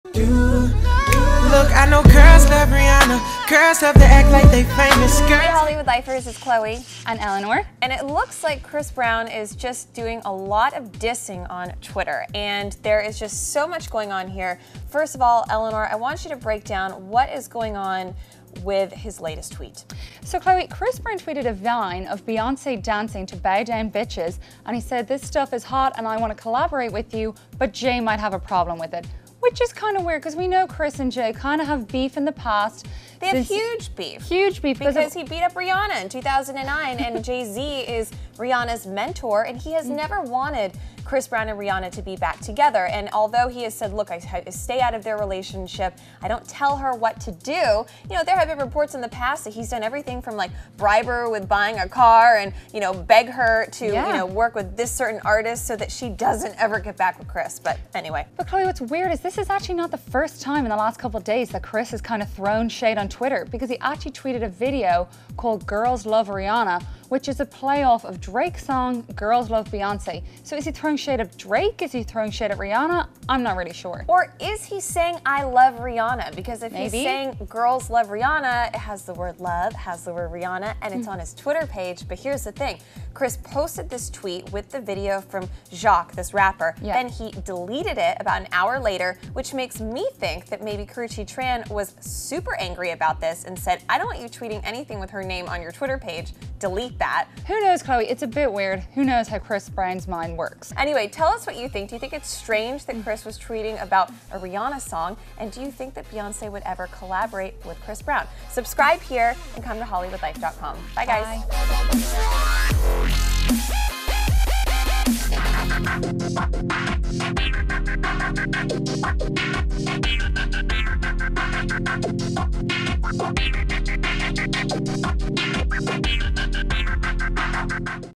Chris Brown Disses Jay-Z & Drake For Beyonce And Rihanna
Chris Brown throws shade at Jay-Z by pitching a collaboration with Beyonce and then disses Drake while giving love to Rihanna.